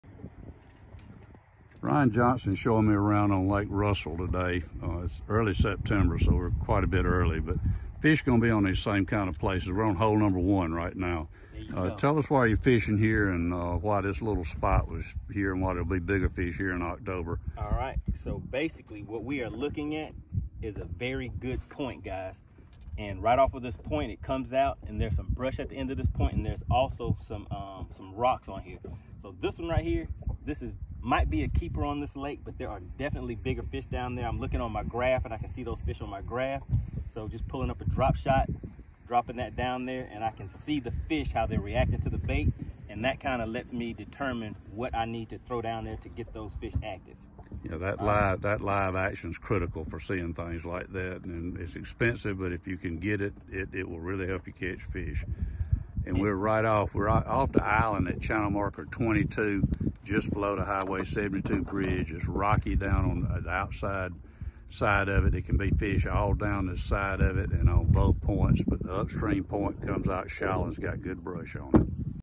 Ryan Johnson Lake Russell Fall Bass
Ryan Johnson took outdoor writer Ronnie Garrison on a tour of Lake Russell on the Georgia-South Carolina line to set an October pattern for spotted bass. Ryan marked a map with 10 GPS locations in a feature article in the October issue of GON and available online to GON subscribers